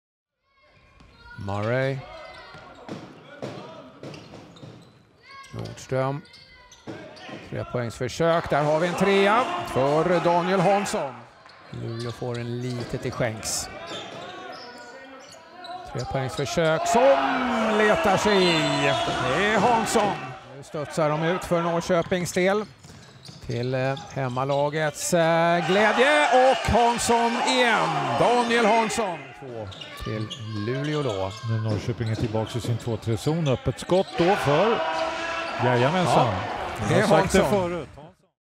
Adecco player of the game vs. Norrköping: Daniel Hansson
Adecco player of the game vs. Norrköping Dolphins: Daniel Hansson.

Hansson gjorde 14 poäng, 2 assists och tog 2 steals.